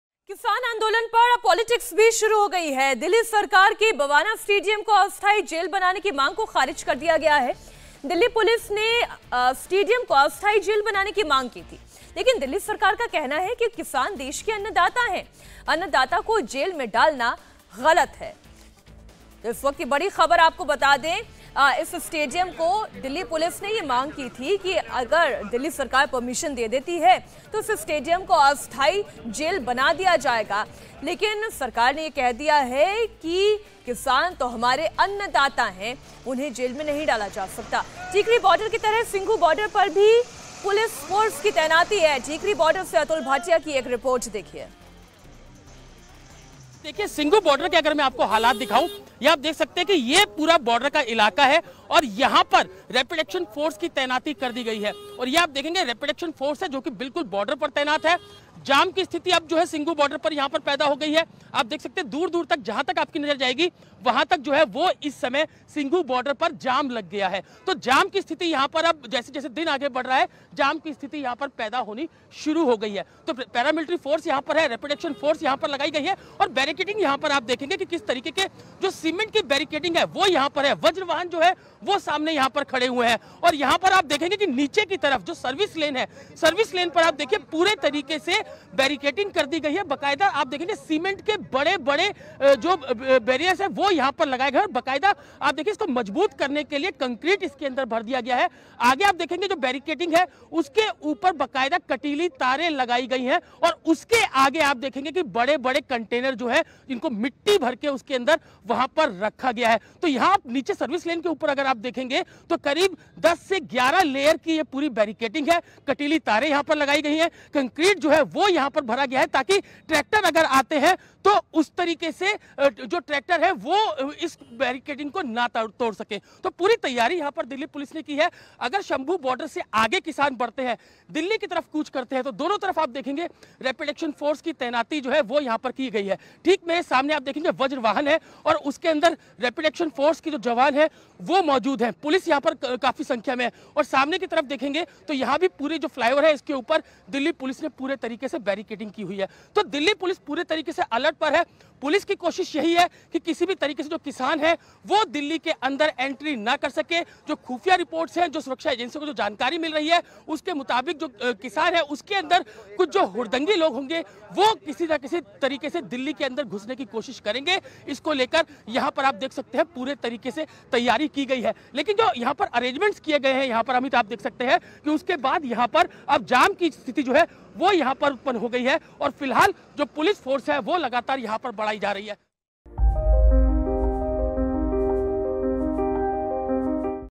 Kisan Andolan 2.0 : किसान आंदोलन पर Kejriwal पॉलिटिक्स , 24 से पहले कैसी ये लड़ाई ? Farmer Protest
Kisan Andolan 2.0 : किसान आंदोलन पर अब पॉलिटिक्स भी शुरू हो गई है. बता दें कि बवाना स्टेडियम को अस्थाई जेल बनाने से दिल्ली सरकार ने  इनकार कर दिया है.